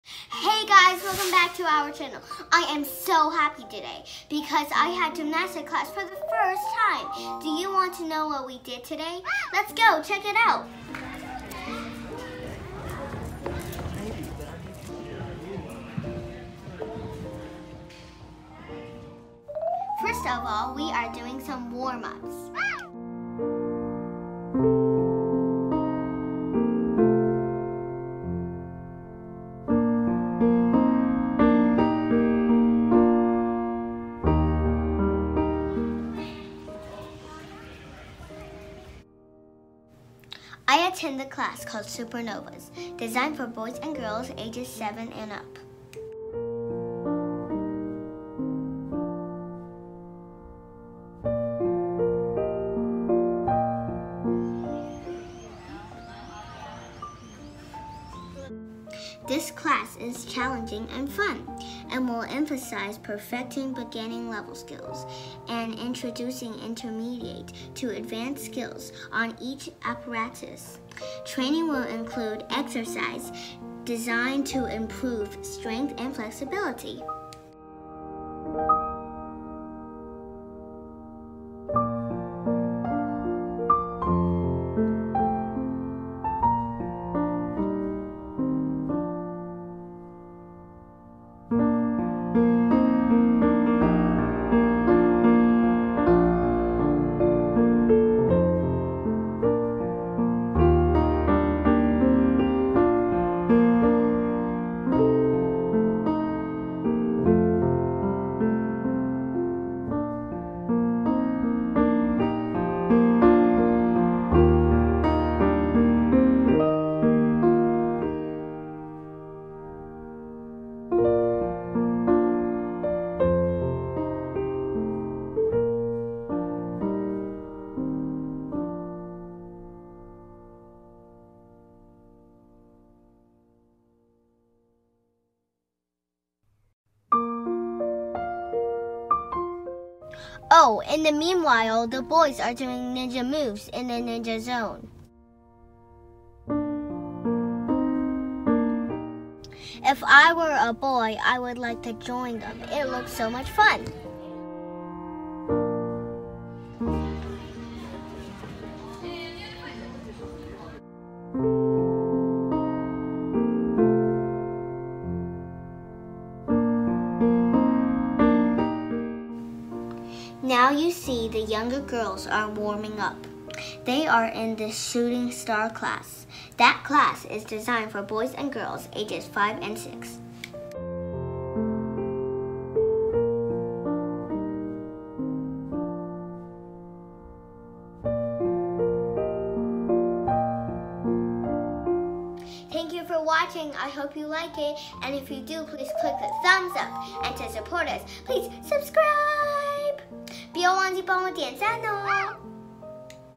My first gymnastic class! What do we do in the 1st gymnastic class? USA. Lily的第一次體操課偶遇Ninja Zone忍者訓練
My first gymnastic class! What do we do in the first gymnastic class? USA.

Lily的第一次體操課很好玩！偶遇Ninja Zone忍者訓練課！